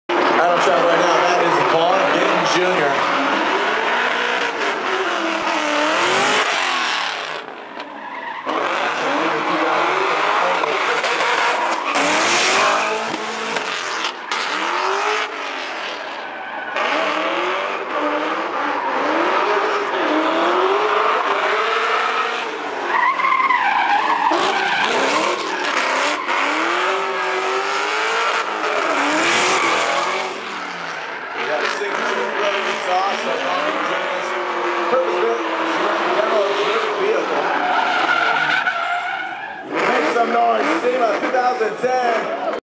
Ken Block with Vaughn Gittin Gymkhana SEMA 2010
Gymkhana footage @SEMA 2010 Tires boiled off high powered mustang